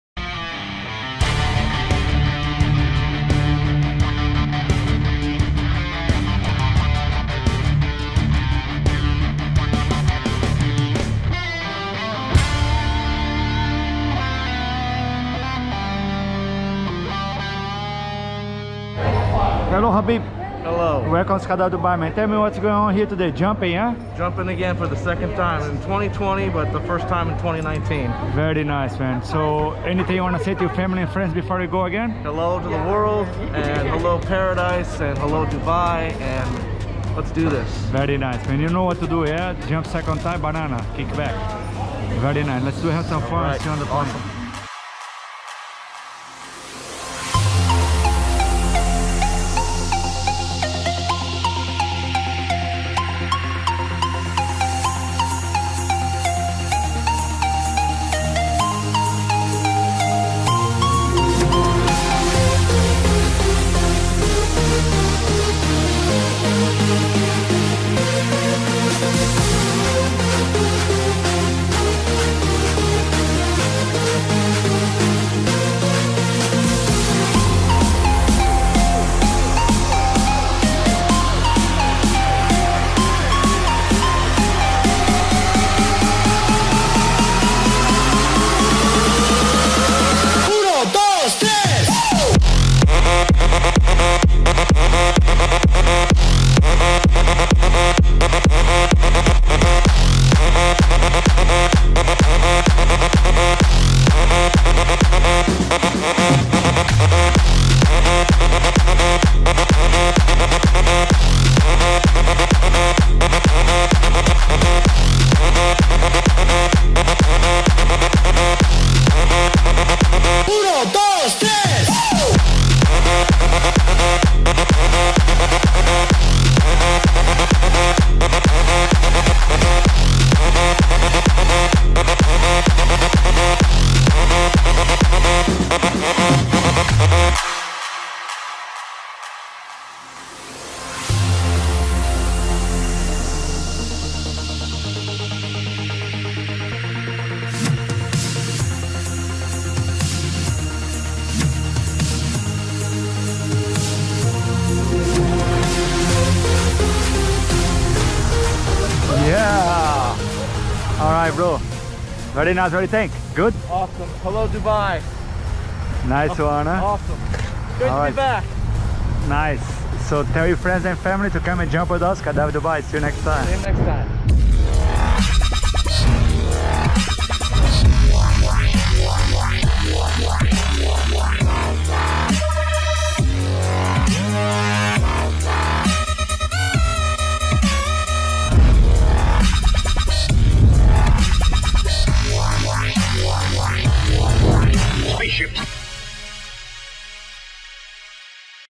SYKDIVE DUBAI 2020
Habeeb Eldabh's 3rd Skydive in Dubai.